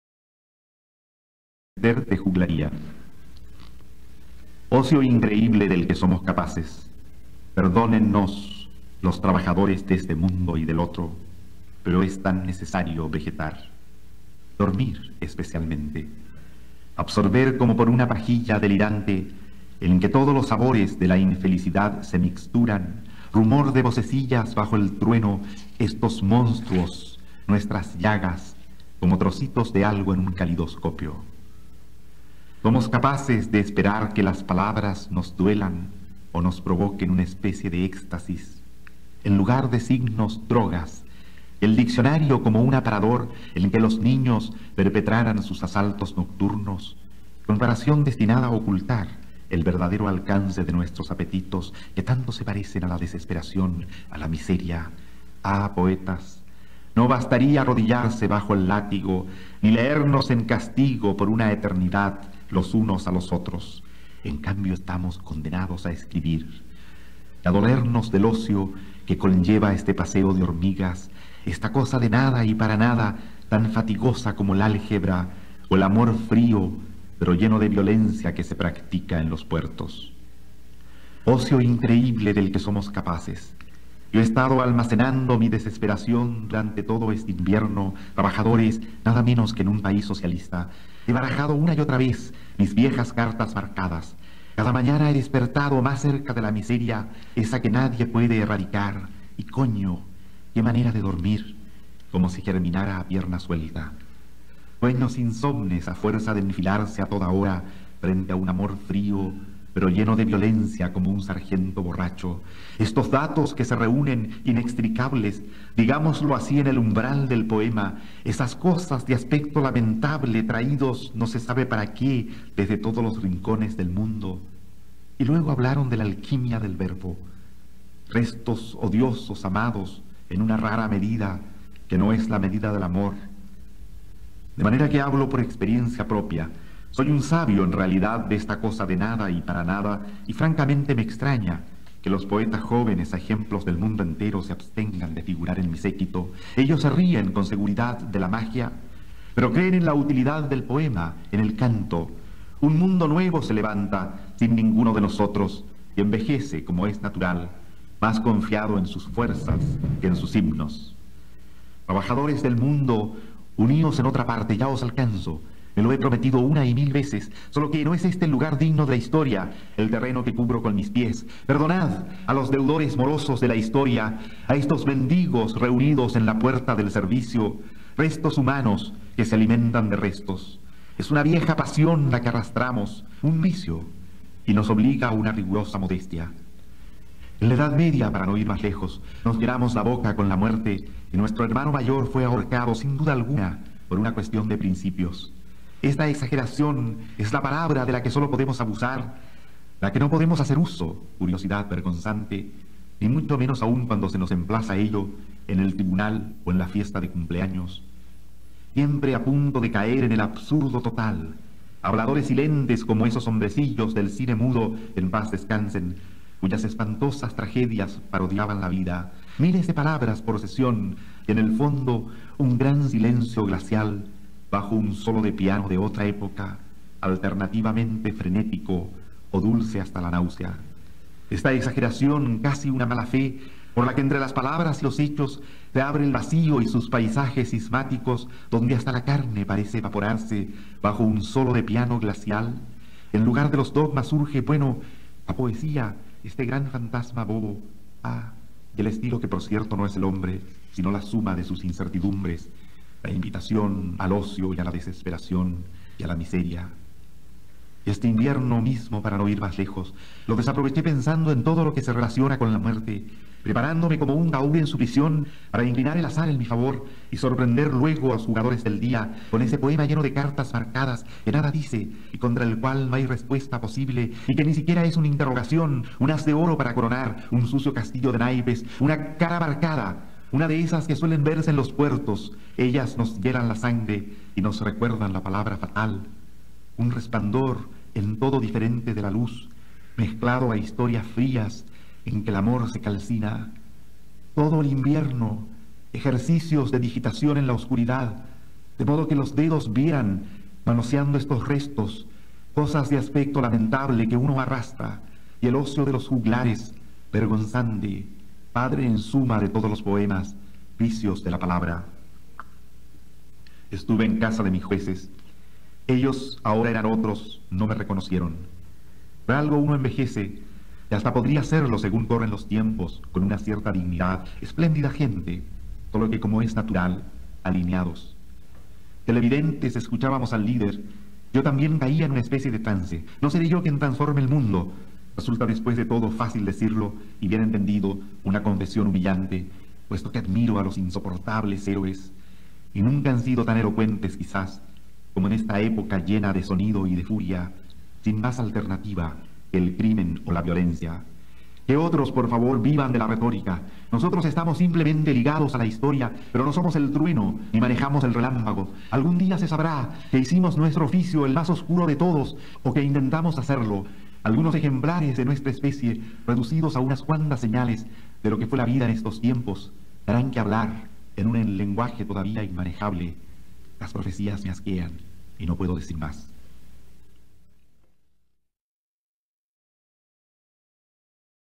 Enrique Lihn: Mester de juglaría poema en voz del autor (poesía chilena)
Poema con audio de Enrique Lihn
Poeta chileno de la generación del 50

Páginas